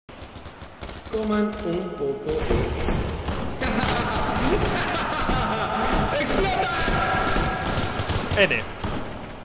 ElRichMC dice pn
¿Escucharon lo mismo? ElRichMC momento XD en el evento de Farfadox Hardcore Z 3